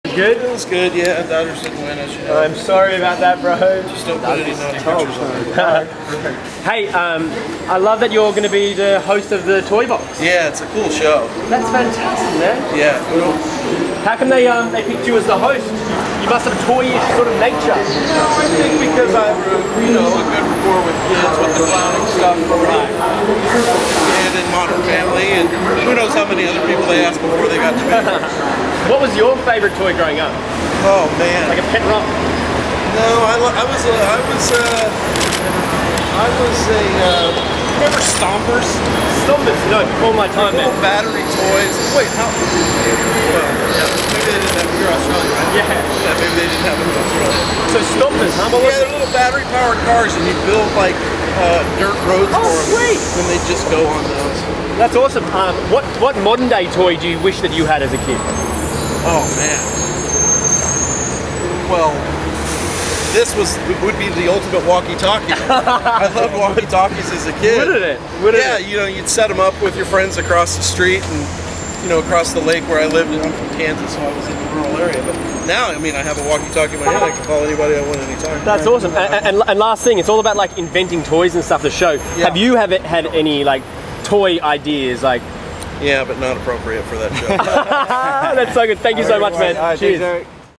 Eric Stonestreet Chats About New Show 'The Toy Box' At LAX
Monday, October 24, 2016 - Eric Stonestreet of Modern Family arrives at LAX after being named host of ABC's new show, 'The Toy Box'. When asked if he's had any toy ideas of his own the funnyman responds, "Yes, but not appropriate for the show!"